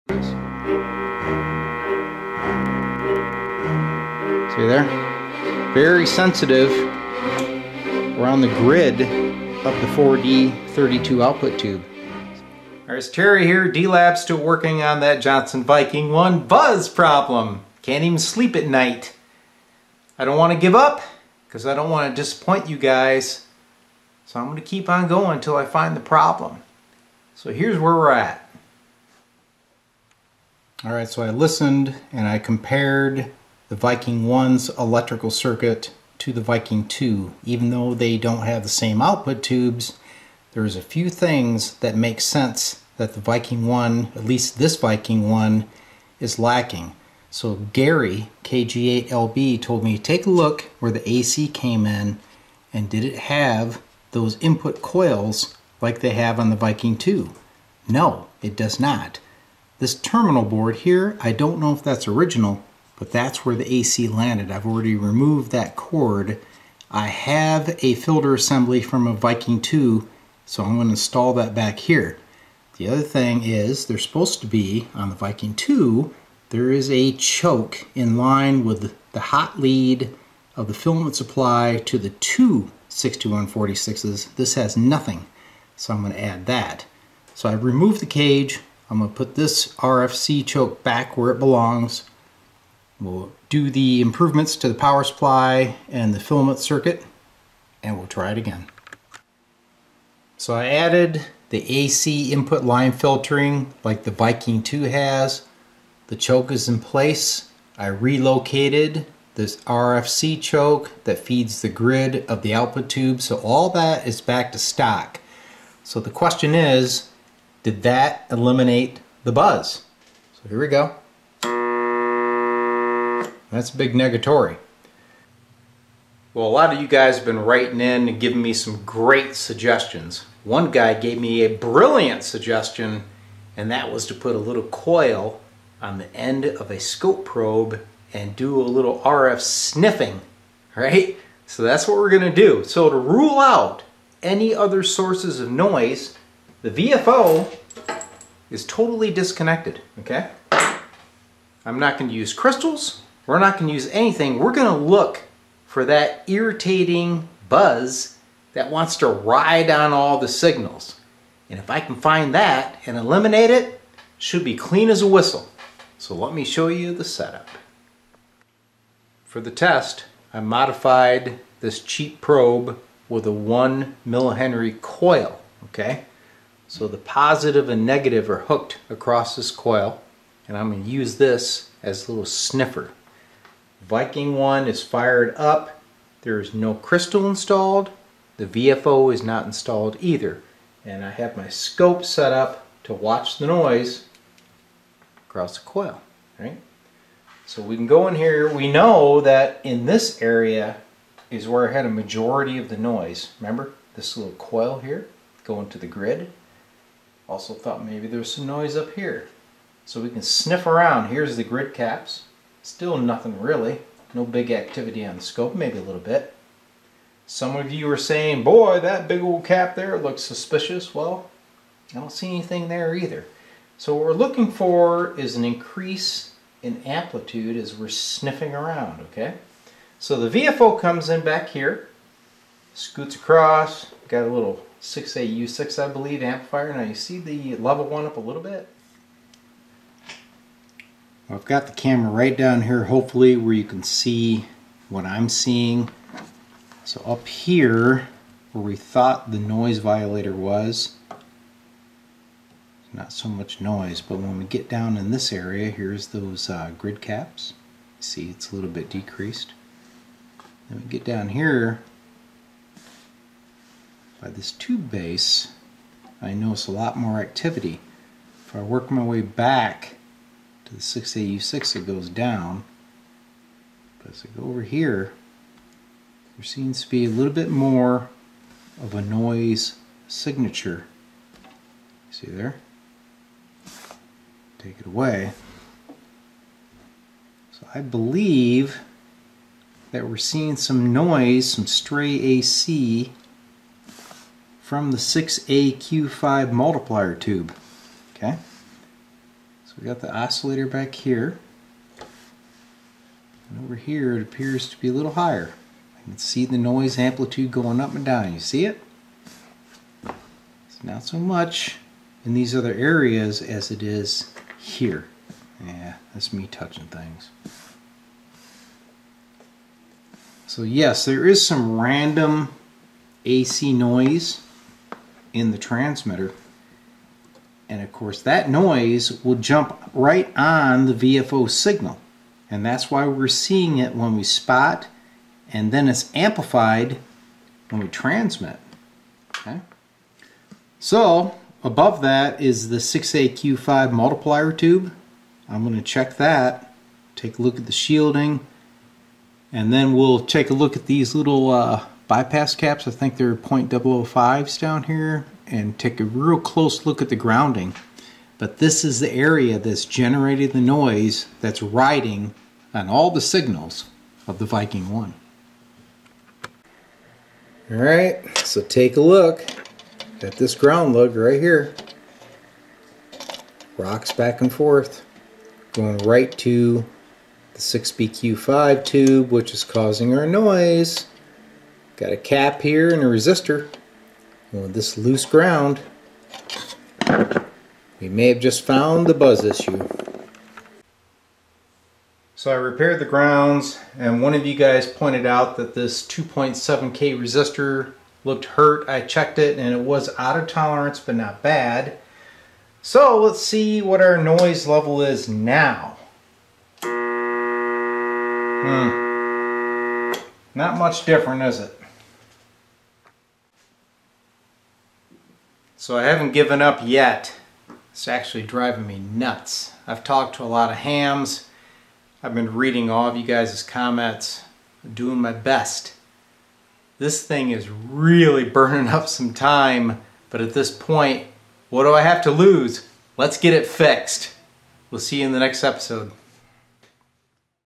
Troubleshooting Johnson Viking 1 Ham Transmitter noise tracing down multiple issues
A follow up video. I appreciate everyone's input, it has really helped to get me thinking. Sometimes I just look for the common issues, not going outside of the D-lab box. Yes, still attempting to clean up the transmission of this old tube rig. Not having much luck. Running out of ideas. I thought I found it with a loose ground, but nope, just playing with me. Really need to put on the thinking cap for the next round. Thanks again everyone for being part of the troubleshooting team. Kind of fun huh?